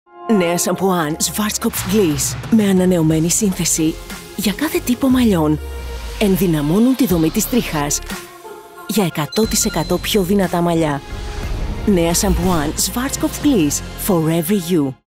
+100% πιο δυνατά μαλλιά* με τα ΝΕΑ Σαμπουάν Schwarzkopf Gliss.
ΝΕΑ ΣΑΜΠΟΥΑΝ GLISS ΑΠΟ ΤΟ SCHWARZKOPF
- Με ανανεωμένη σύνθεση για +100% πιο δυνατά μαλλιά*
- Ενδυναμώνουν τη δομή της τρίχας
- Καθαρίζουν σε βάθος χωρίς να βαραίνουν τα μαλλιά
- Για κάθε τύπο μαλλιών

*σε σύγκριση με μαλλιά που δεν έχει εφαρμοστεί το προϊόν